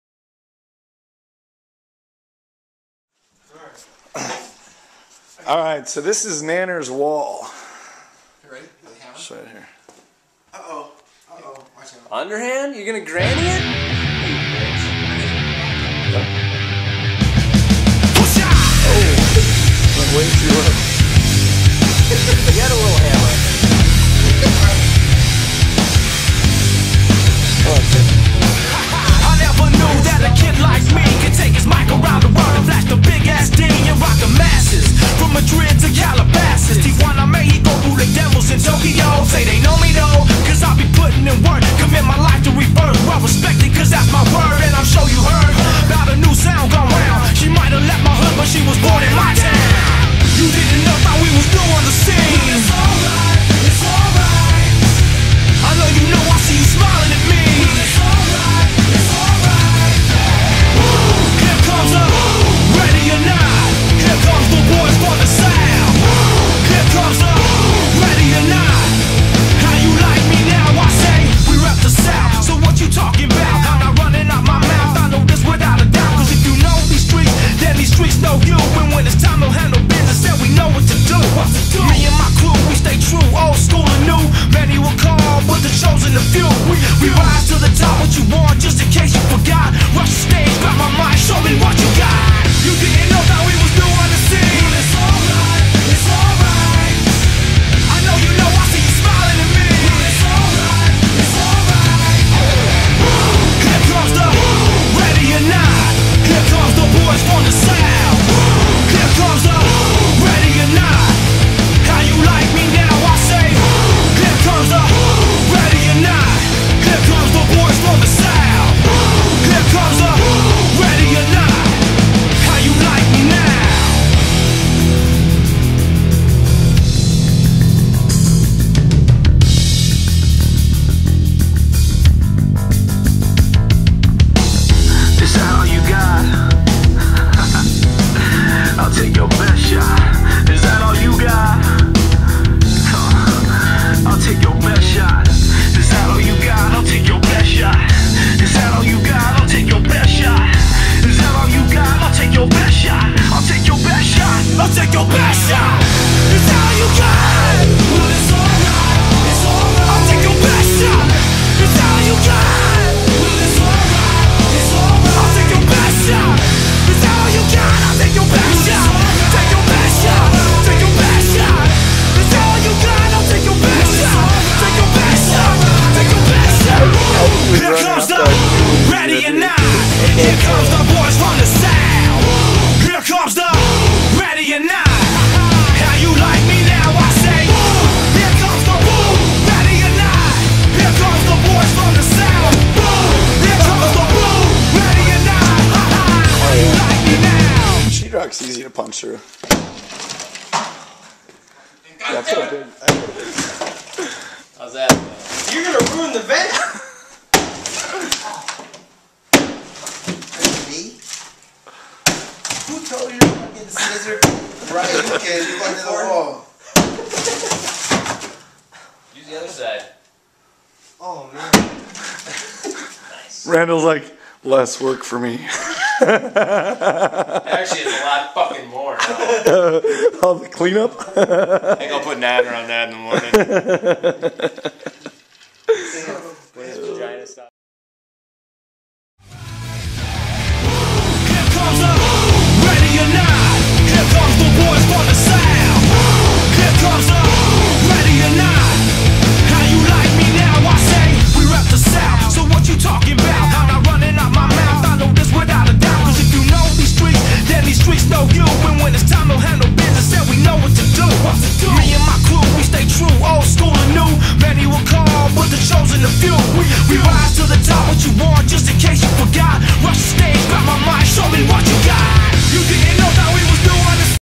breaking down the wall.mov
music video to my friends and i destroying the wall in his kitchen